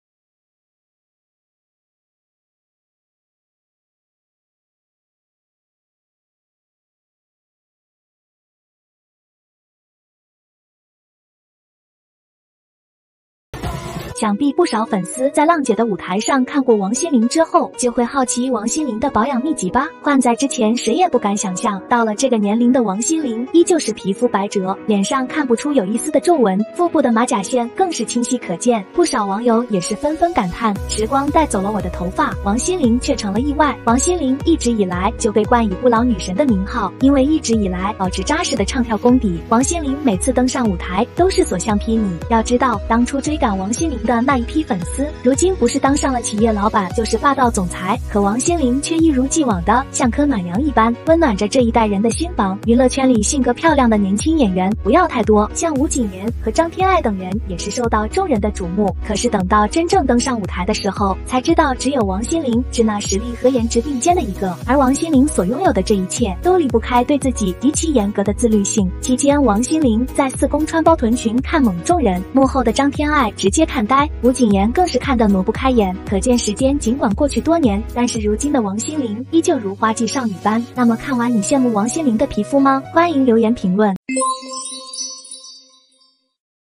王心淩 四公比賽包臀裙看懵眾人，張天愛直接看呆，吳謹言挪不開眼
王心淩四公比賽包臀裙看懵眾人，張天愛直接看呆，吳謹言挪不開眼